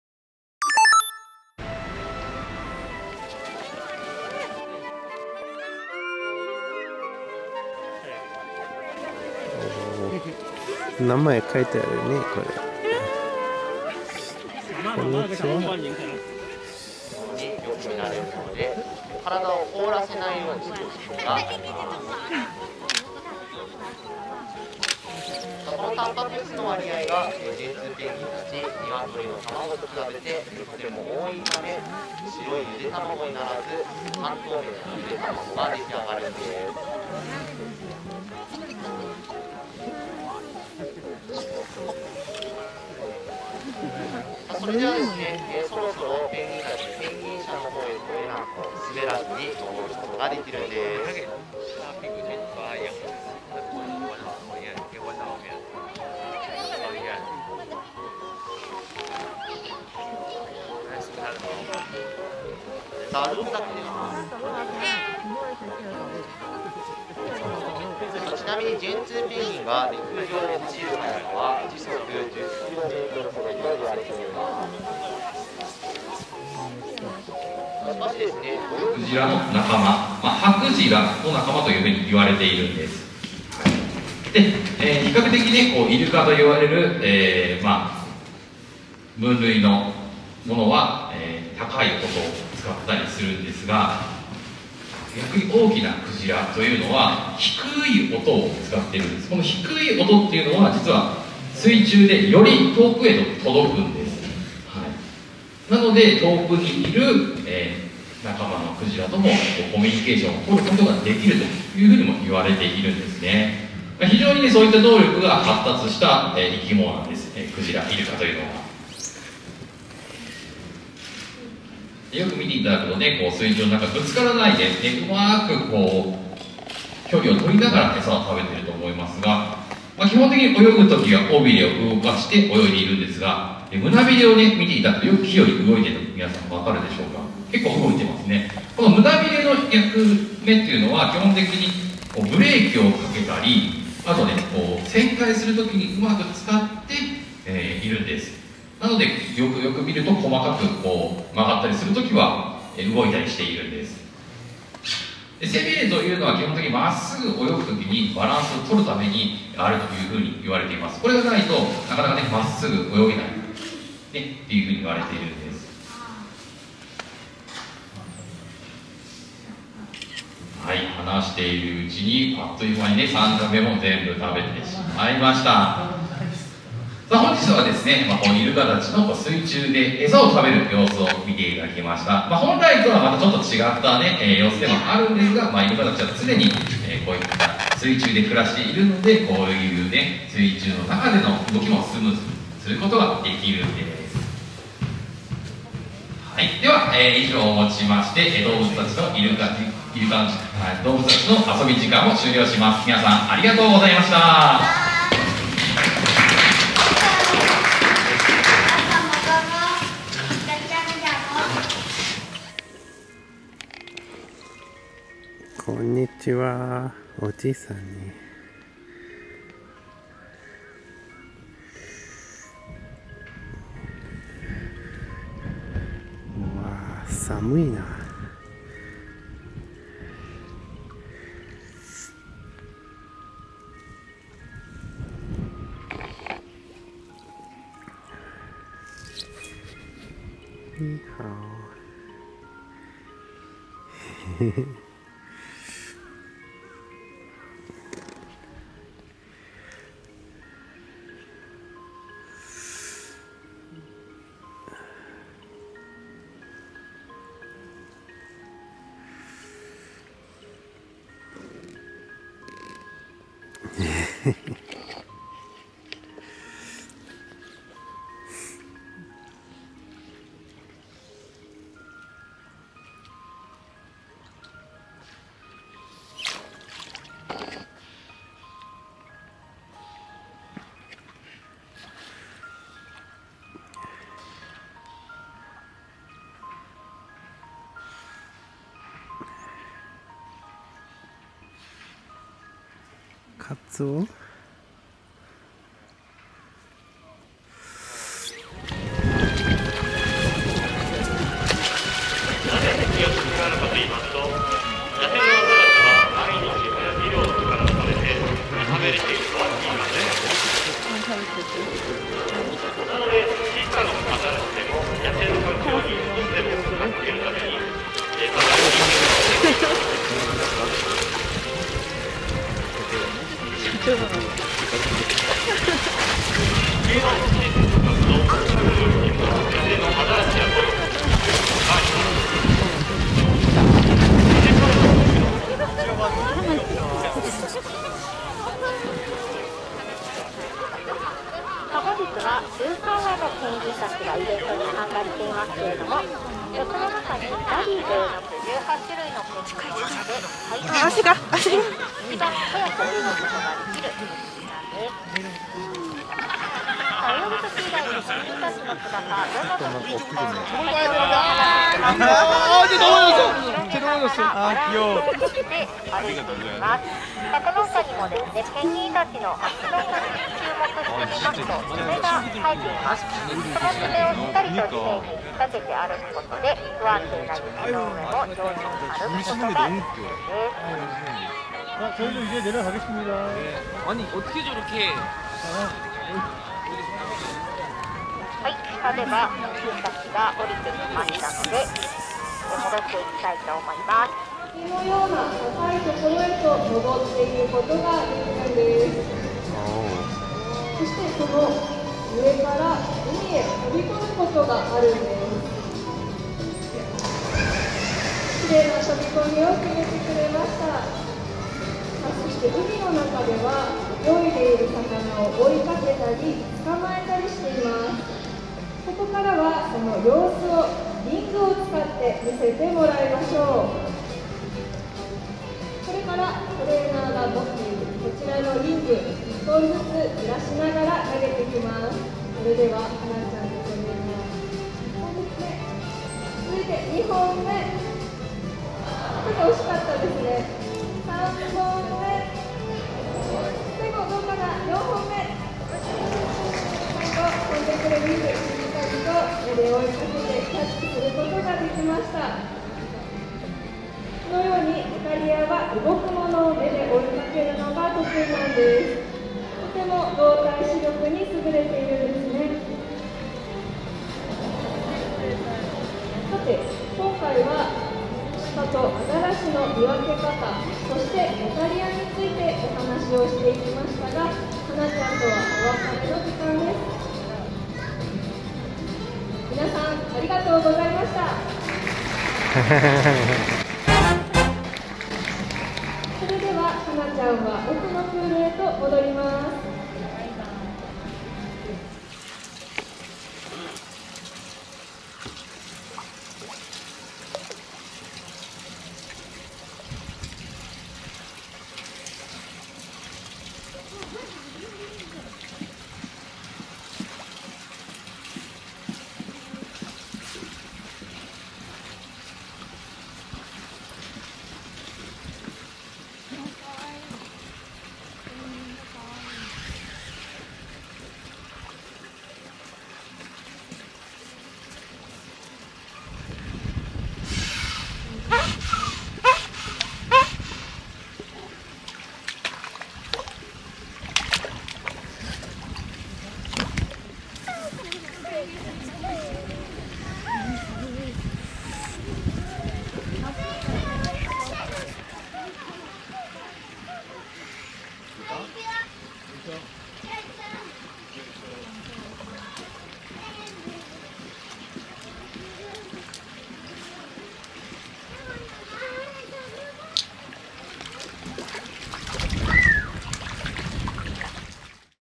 2023.02 北海道大樽水槽館
札幌に近い大樽水槽館でペンギンの散歩、イルカの餌やりなどを楽しみました。
カメラ：ZV -E10